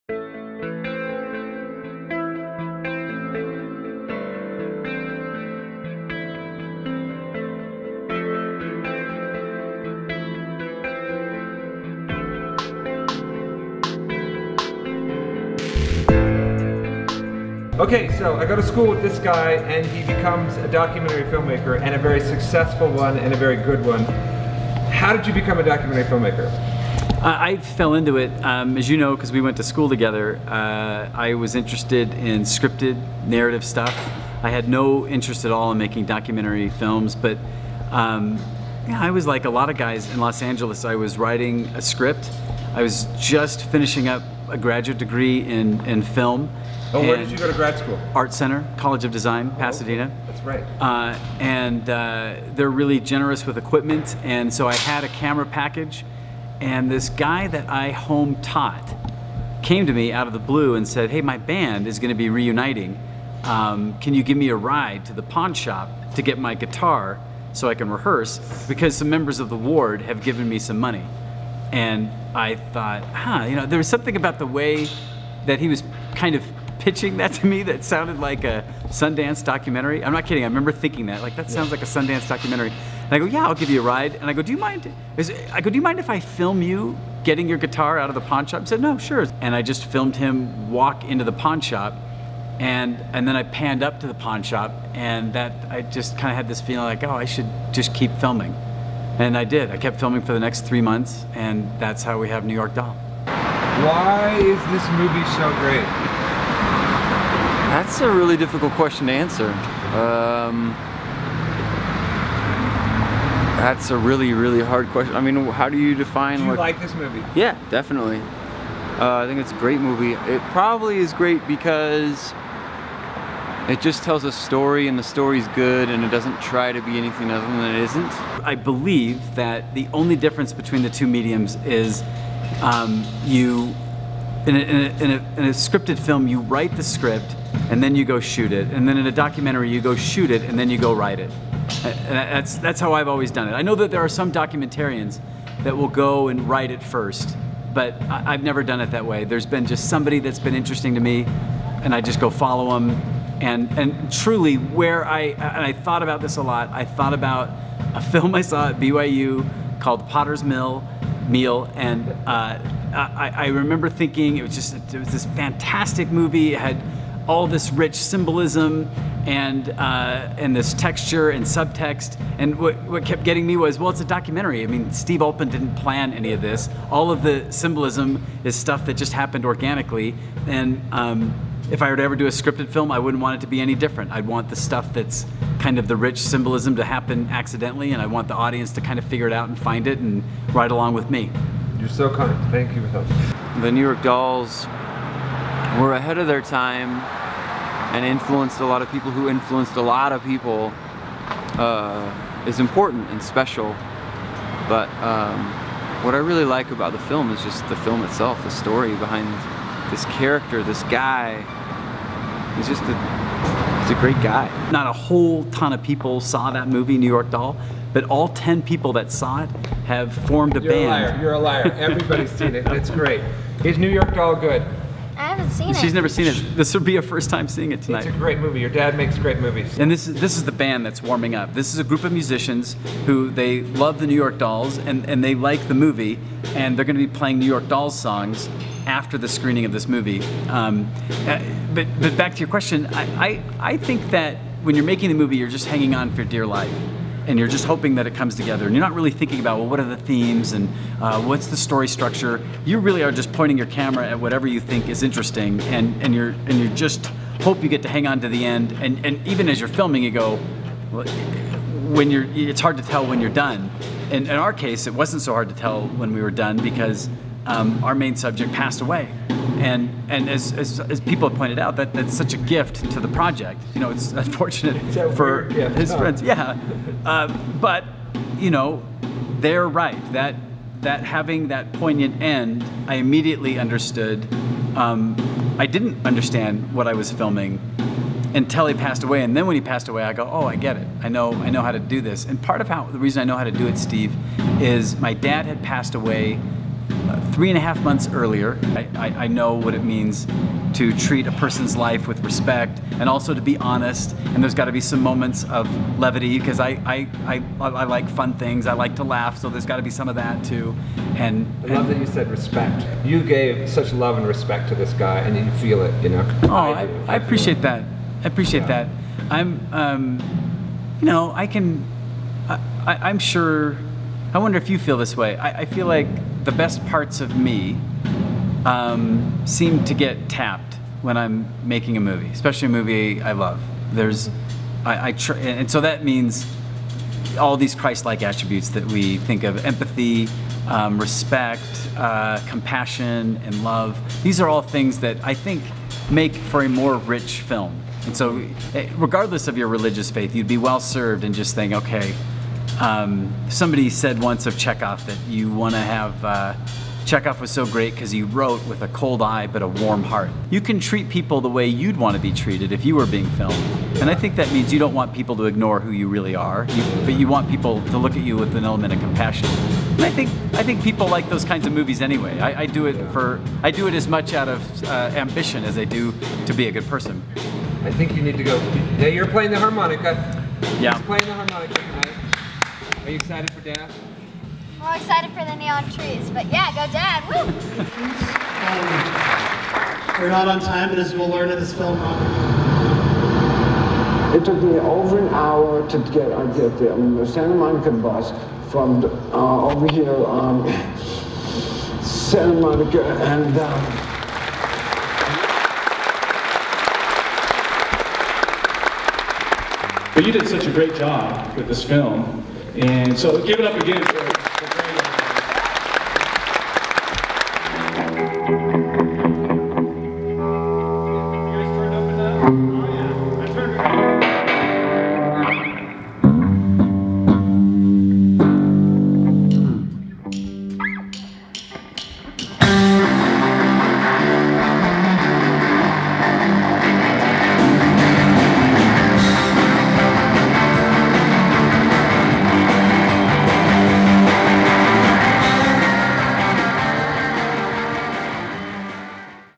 How to Become a Documentary Filmmaker: New York Doll
A conversation with documentary filmmaker Greg Whiteley on the tenth anniversary of his film New York Doll. Greg and I went to film school together. We talk about the film and the idea of being a documentary filmmaker, Enjoy!

Check out Greg's films at onepotatoproductions.com